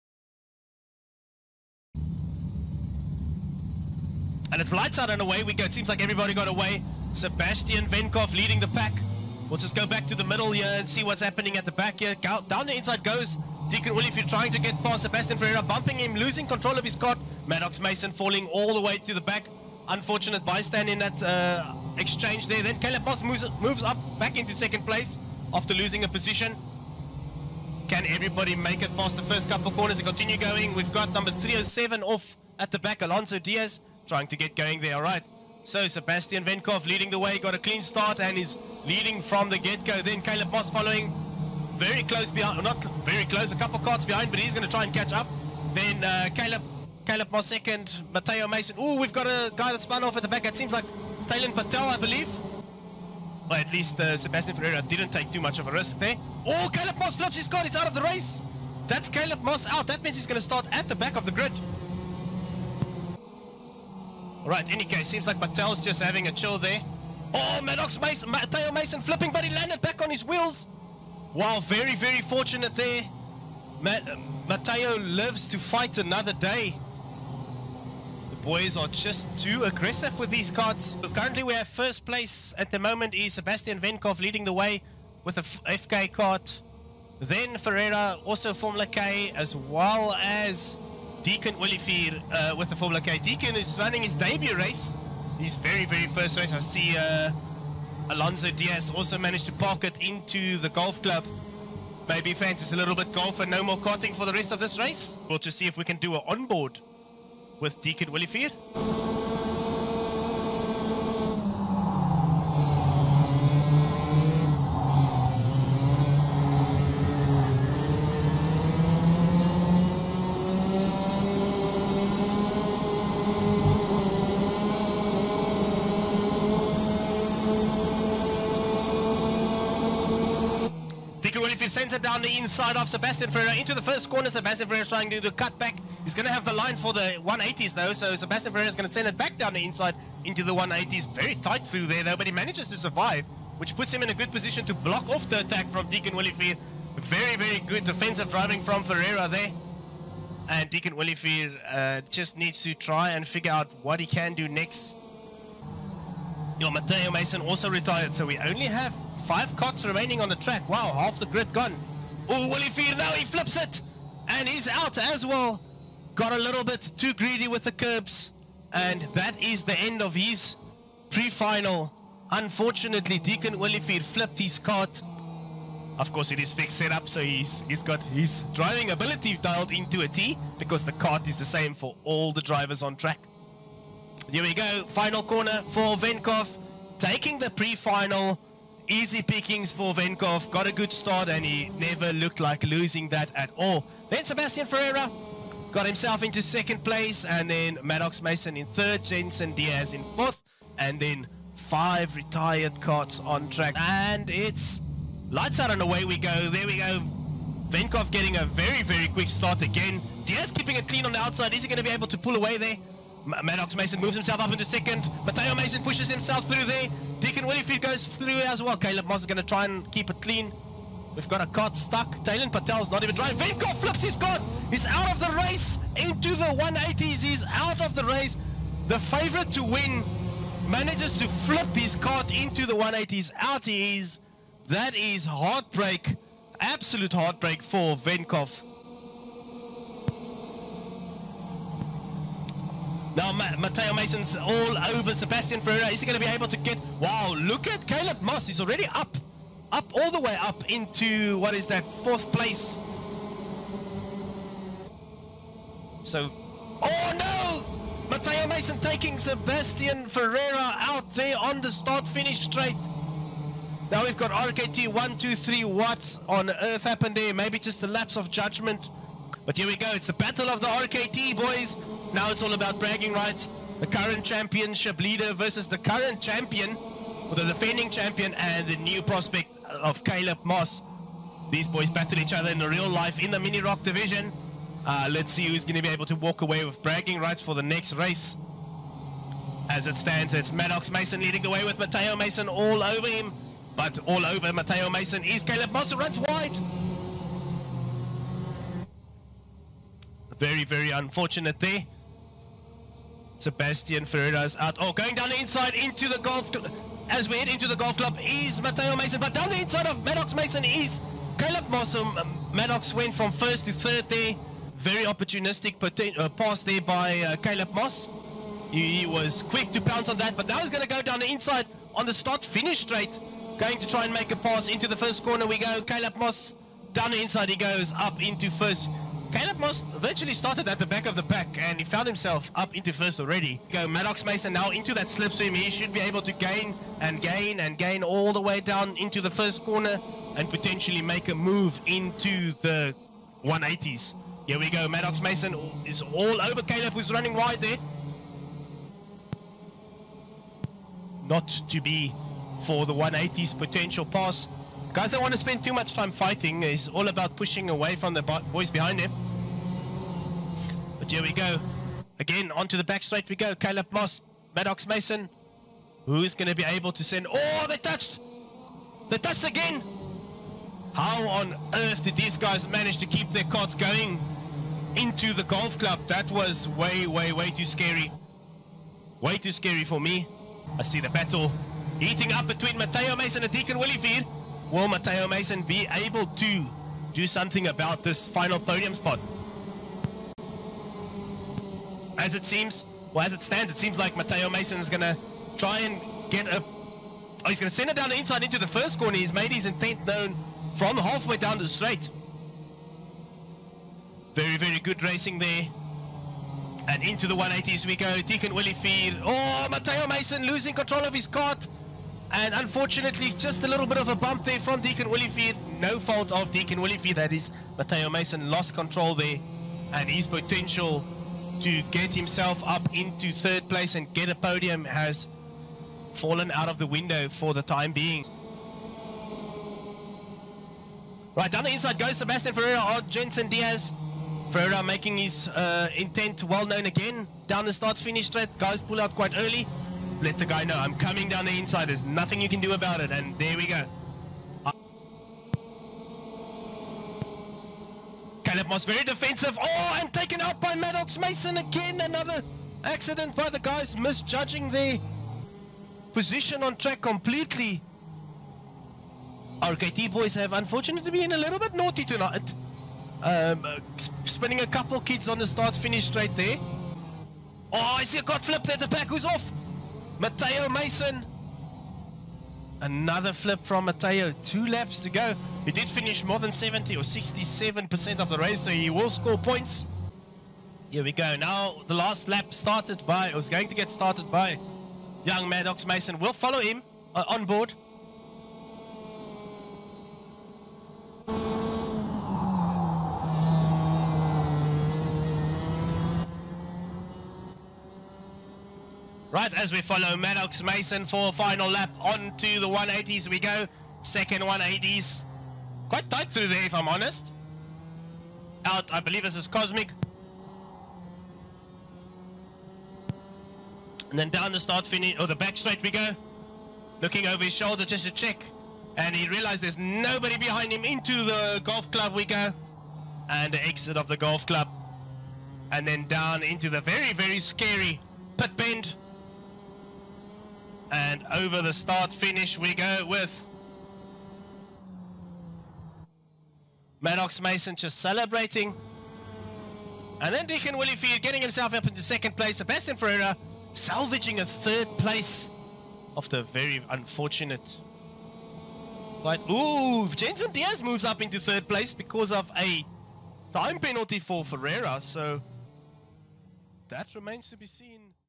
OKLA Kart Racing Pro Season 4 Round 3 Juniors | Killarney
Catch up with all the action from our second Pro League event from OKLA Season 4 at Killarney